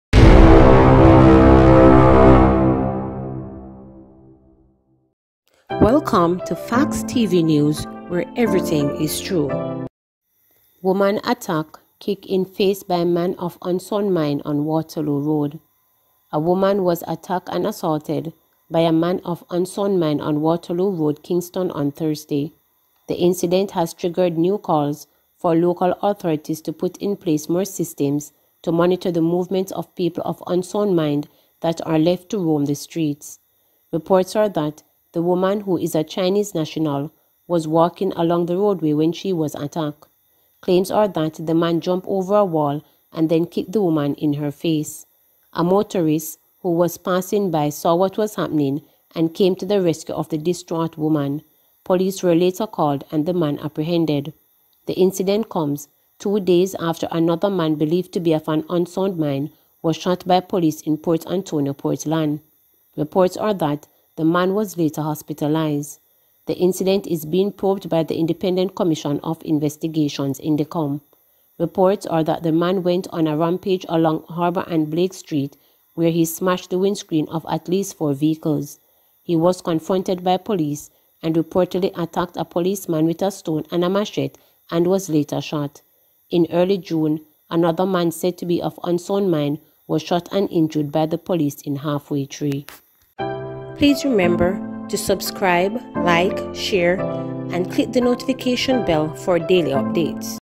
Mad Man Kick Chinese Woman In Face On Waterloo Road Jamaica
woman attacked and kick in face by man of unsound mind